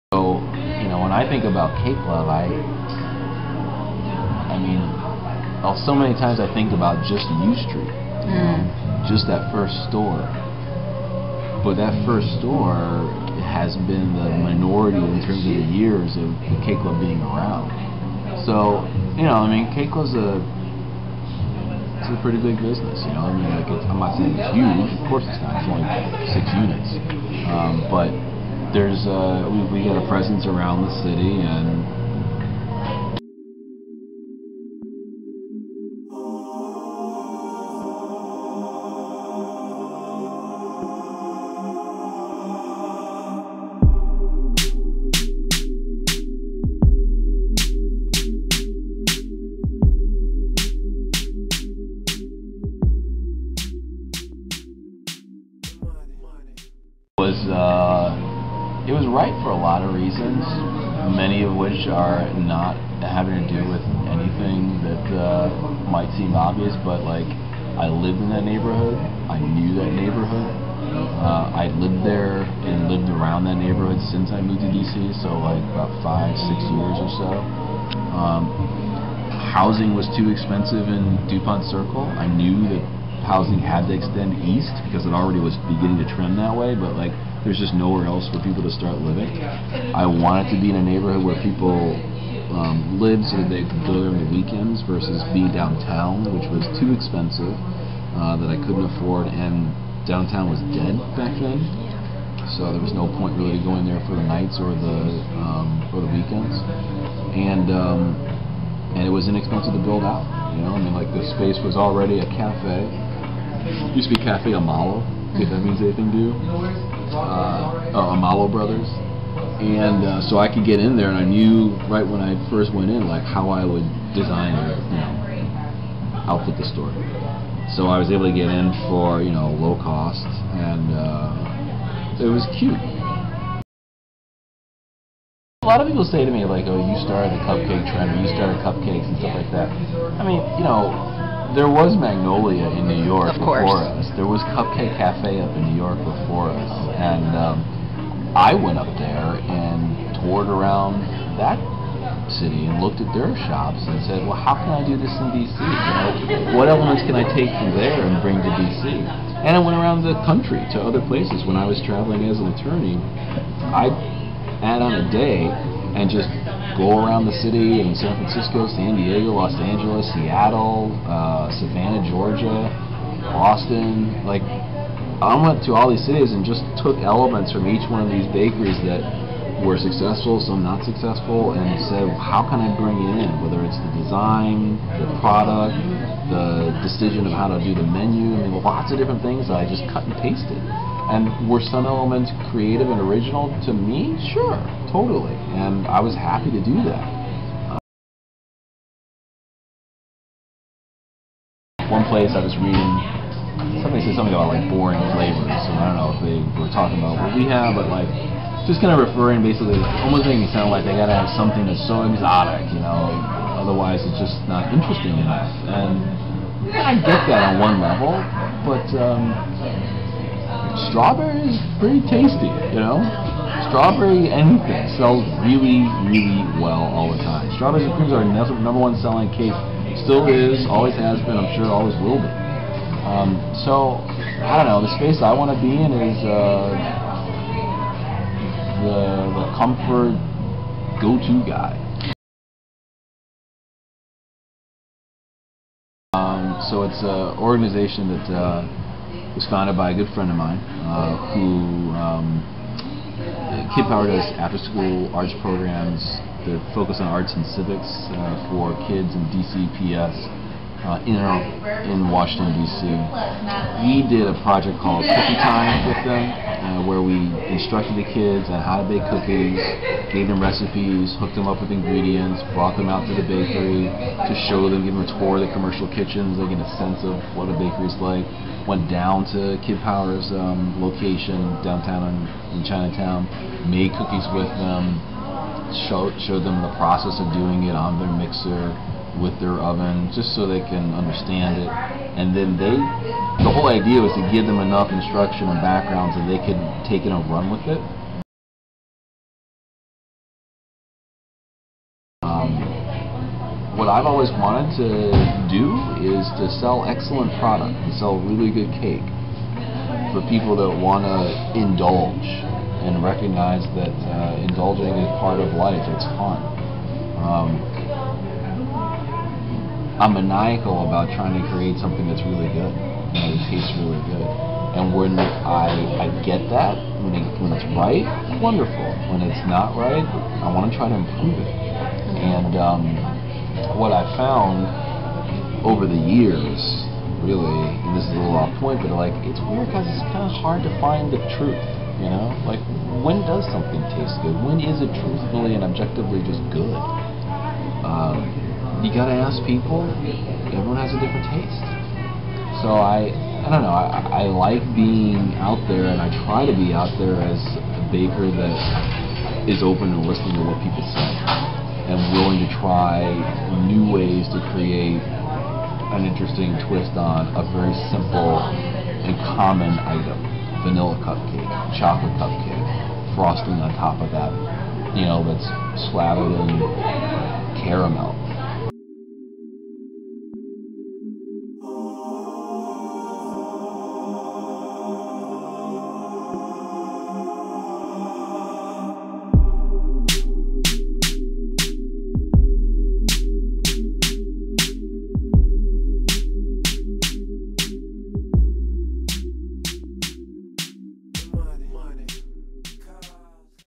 Meet The Baker: Warren Brown of CakeLove (Pt.1)
The Cupcake Critic presents a new series called, Meet The Baker. Our first entry is Warren Brown, owner of CakeLove. Brown, a former attorney, is the author of three books and the host of the Food Network's, Sugar Rush. His bakery, CakeLove currently has 6 locations throughout D.C. and has been highlighted by Dell, American Express and Oprah Winfrey.  I sat down with Brown at his downtown Silver Spring store to discuss: business, parenthood, his challenges and triumphs.